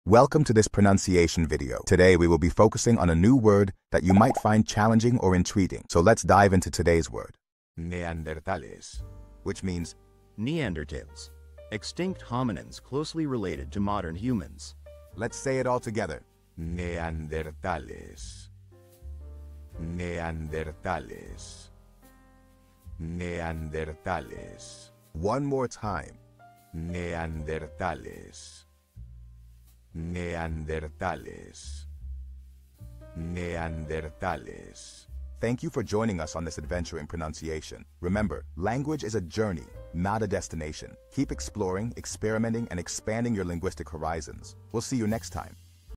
How to pronounce Neandertales
Master the Pronunciation of 'Neandertales Which Means Neanderthals' - which means : Neanderthals: Extinct Hominins Closely Related to Modern Humans. 📝 with @PronunciationSpanish 🎓🗣️ - [Your Guide to Spanish]

Learn to pronounce 'Neandertales' perfectly in Spanish with our easy and engaging tutorial by @PronunciationSpanish! 📖📽️ In this comprehensive video, we provide you with native speaker examples 🗣️👂 and phonetic breakdowns 🎵 to ensure you get the pronunciation just right.

How to say Neanderthals in Spanish